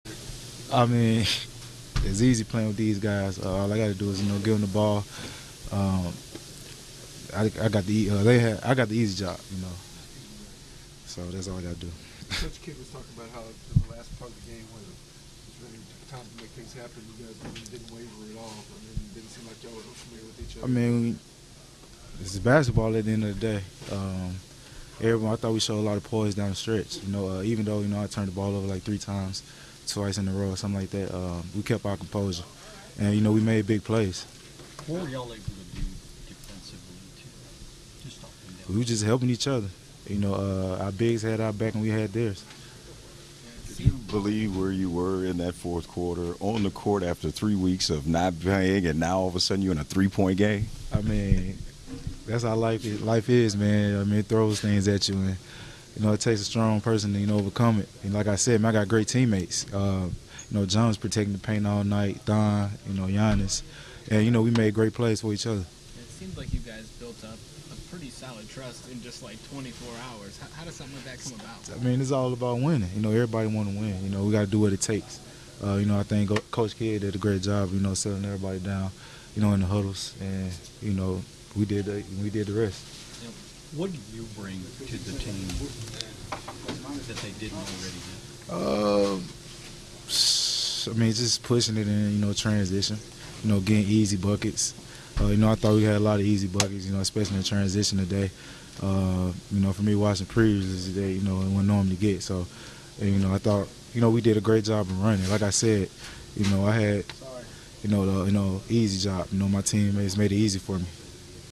Bledsoe on his Bucks debut: 'It's easy playing with these guys'
After being traded from the Phoenix Suns to the Milwaukee Bucks, Eric Bledsoe says he's fitting in just fine.